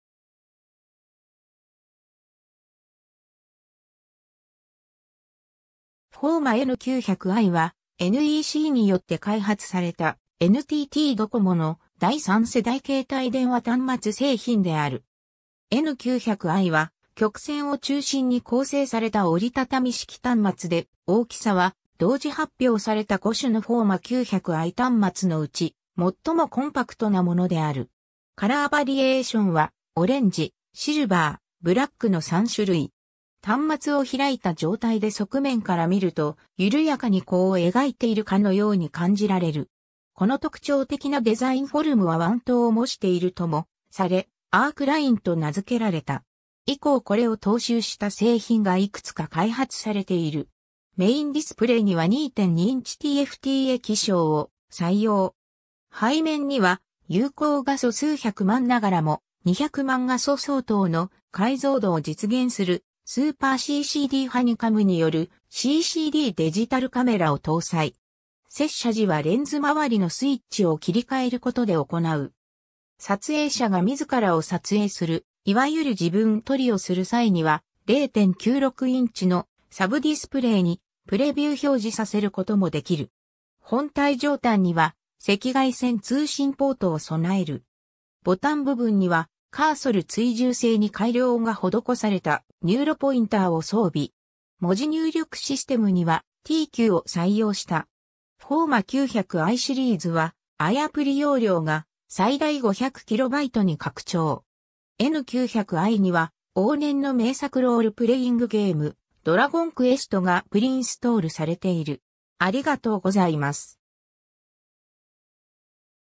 N900i
携帯電話端末_(日本電気)
FOMA N900i（フォーマ・エヌ きゅう まる まる アイ）は、NECによって開発された、NTTドコモの第三世代携帯電話 (FOMA) 端末製品である。
 N900iは、曲線を中心に構成された折りたたみ式端末で、大きさは同時発表された5種のFOMA 900i端末のうち最もコンパクトなものである。
 カラーバリエーションはオレンジ、シルバー、ブラックの三種類。
 端末を開いた状態で側面から見ると、ゆるやかに弧を描いているかのように感じられる。
 この特徴的なデザインフォルムは彎刀を模しているともされ、アークラインと名付けられた。
 以降これを踏襲した製品がいくつか開発されている。
 メインディスプレイには2.2インチTFT液晶 (QVGA) を採用。
 背面には有効画素数100万ながらも、200万画素相当の解像度を実現するスーパーCCDハニカムによるCCD デジタルカメラを搭載。
 接写（マクロ撮影）時はレンズ周りのスイッチを切り替えることで行う。
 撮影者が自らを撮影する、いわゆる自分撮りをする際には、0.96インチのサブディスプレイにプレビュー表示させることもできる。
 本体上端には赤外線通信ポートを備える。
 ボタン部分にはカーソル追従性に改良が施されたニューロポインターを装備。
 文字入力システムにはT9を採用した。
 FOMA 900iシリーズはiアプリ容量が最大500キロバイトに拡張。
 N900iには、往年の名作ロールプレイングゲーム (RPG) 、『ドラゴンクエスト』がプリインストールされている。